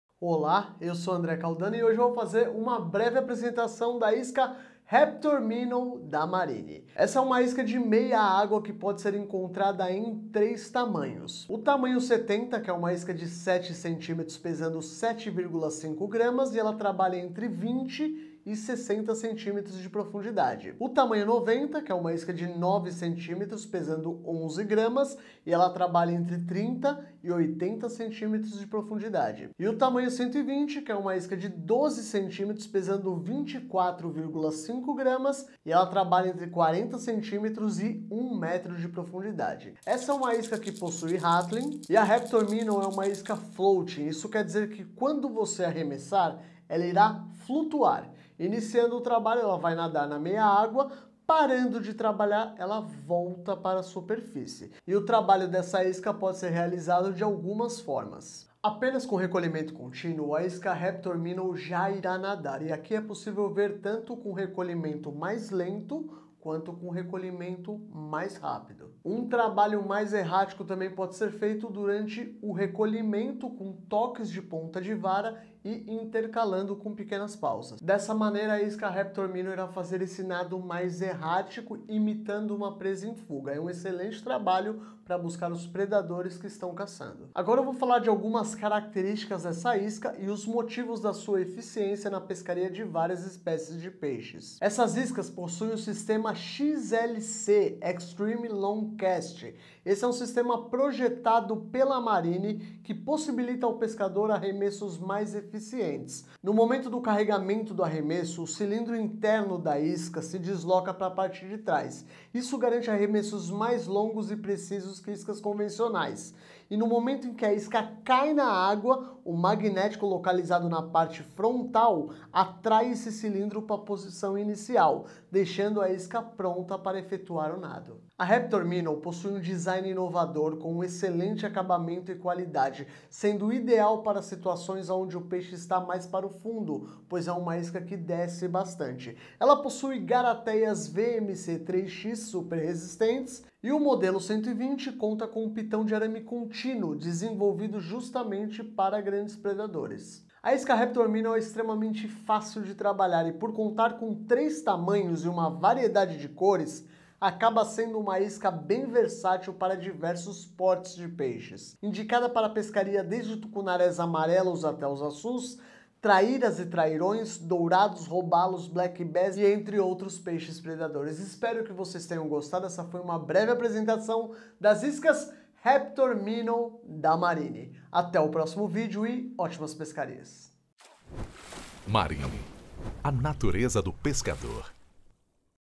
Isca Artificial Marine Sports Raptor Minnow 90 - 9cm 11g
Da marca Marine Sports, a isca Raptor Minnow 90 possui um sistema magnético XLC eXtreme Long Cast.
Durante o arremesso as esferas de aço desprendem-se da trava magnética se deslocando para a extremidade da isca aumentado o alcance e a precisão do arremesso. No momento em que a isca cai na água ela volta para a posição correta, garantindo o nado perfeito.

Especificações:

Fabricante: Marine Sports
Modelo: Raptor Minnow 90
Ação: Meia água (Floating)
Tipo: Nado errático
Rattlin - Efeito sonoro
Tamanho: 9 cm
Peso: 11 gr
Garateias: VMC 3X